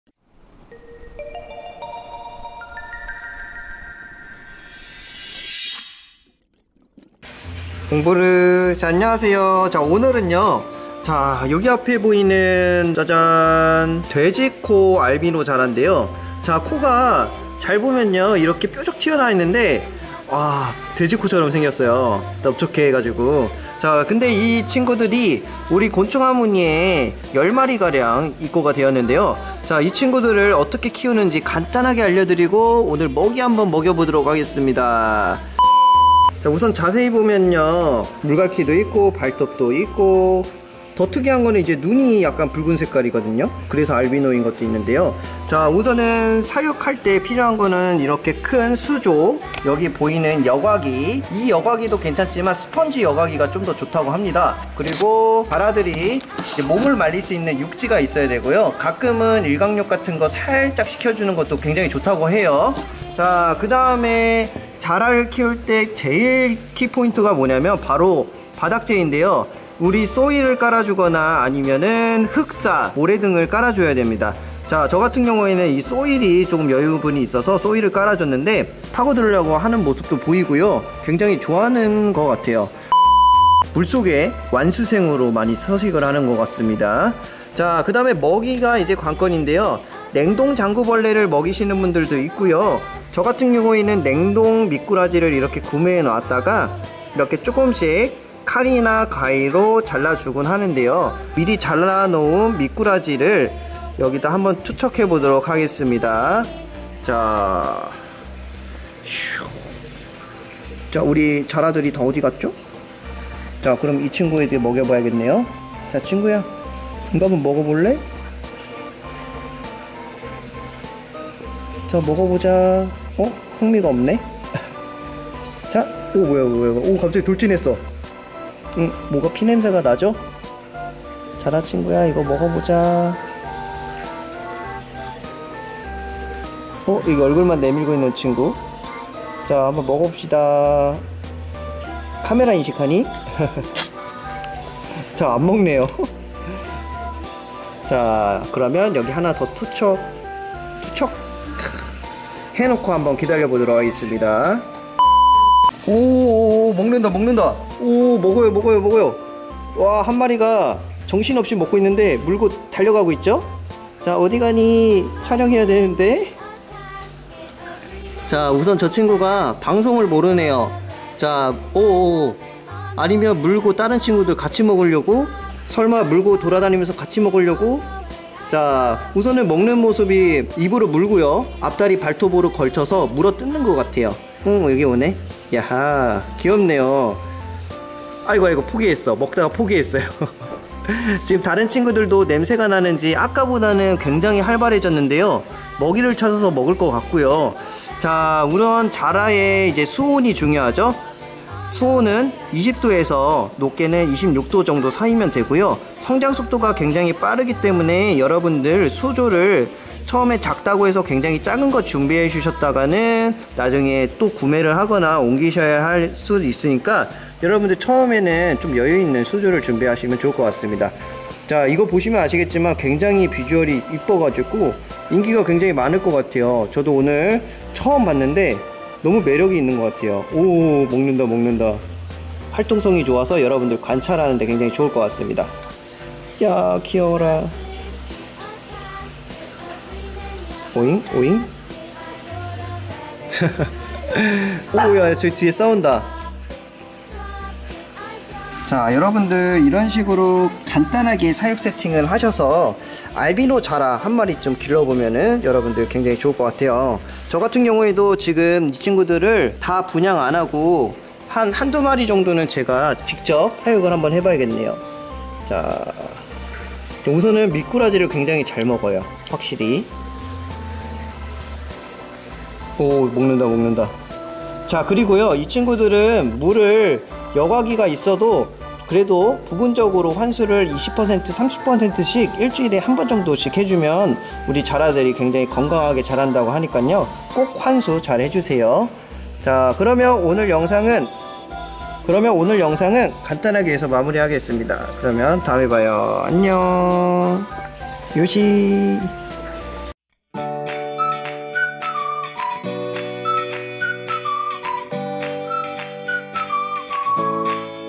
"돼지코 알비노 자라"어떻게 이런게 살아있지...? [정브르]
알비노는 염색체 결핍으로 인한 발색을 가진 개체들을 말합니다.
눈에 빨갛거나 몸의 색이 연한 흰색,살색을 많이 뜁니다.

알비노 자라는 보기힘든 종은 아니고, 국내에서 많이 유통이 되어 많은 브리더들의 사랑을 받고 있습니다.

요번 곤충하모니에도 많이 입고되었는데요.

냉동장구벌레, 냉동 미꾸라지를 먹으며 살아가며, 먹성이 좋고 , 성장속도도 빠르고 생명력이 강한 자라입니다!

한번쯤 꼭 키워보세요!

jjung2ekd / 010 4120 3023

경기도 성남시 분당구 야탑동 361-3번지 금탑프라자 2층